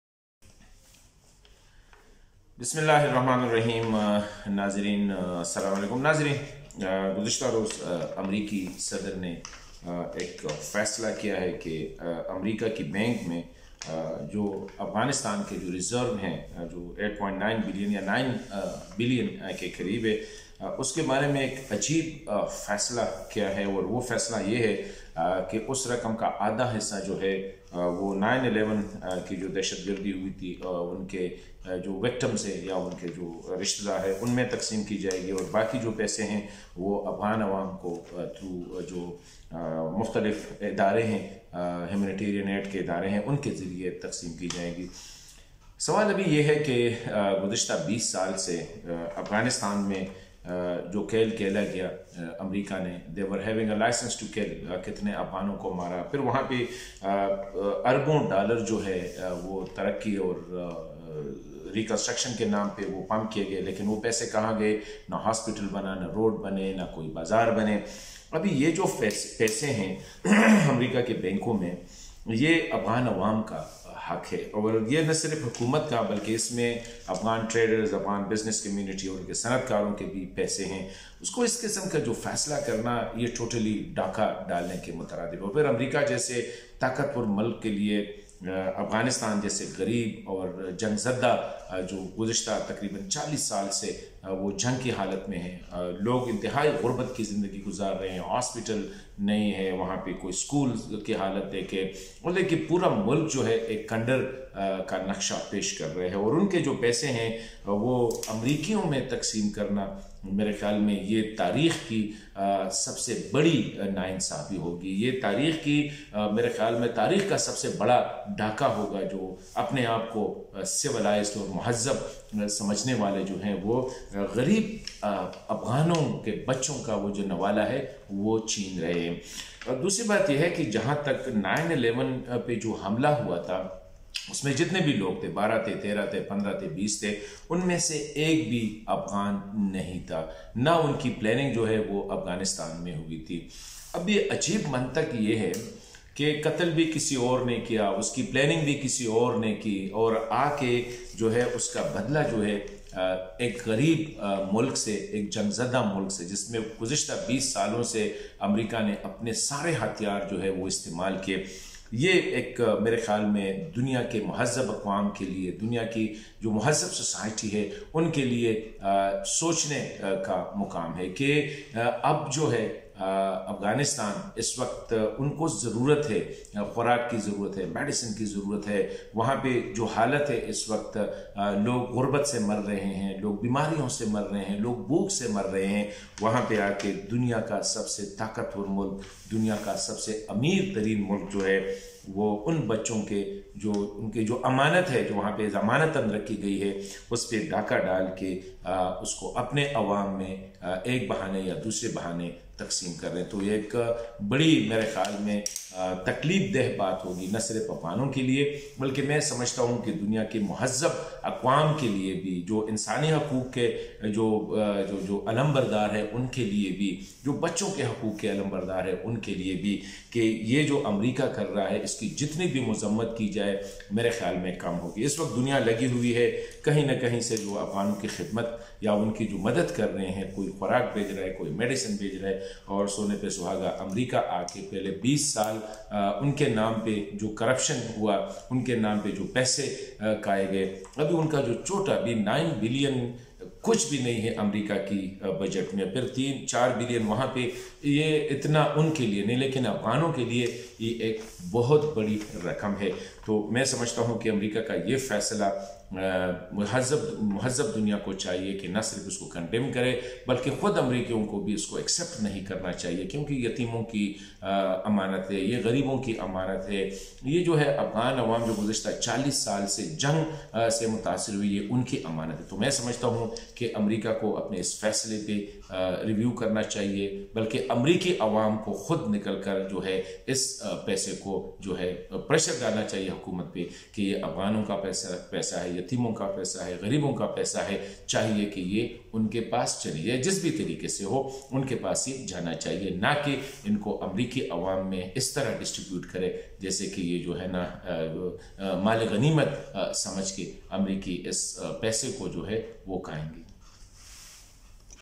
امریکہ کا غریب افغان عوام کے پیسوں پر قبضہ.. امریکیوں کی اخلاقی پستی کا ثبوت
This is indeed quite unfortunate that the world wealthiest nation -American - has claimed the money of the world poorest of the poor nation - the Afghans. Its a fact that the money belong to the afghan people while US is punishing the poor and war devastated nation to take a sheer revenge from Taliban. Its unfortunate